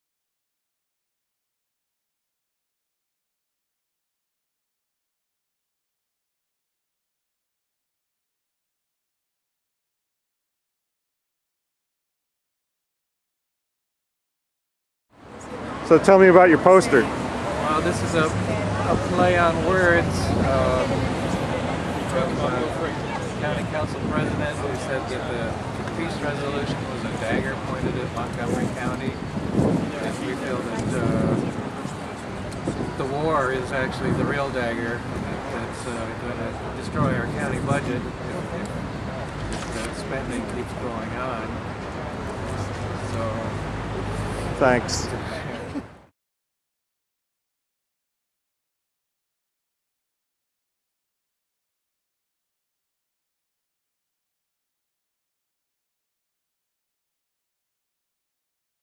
Fund Our Communities supporter Tim Willard speaks before 10/11/11 protest press conference
On Tuesday, October 11, 2011, "Fund Our Communities" coalition members held a rally and press conference on the stairs of the Montgomery County Council building.   The event was held to protest Council's disappointing decision to yield to Lockheed Martin lobbying and shelve consideration of the "Resolution Supporting Federal Budget Reprioritization" advocated by FOC.  Links: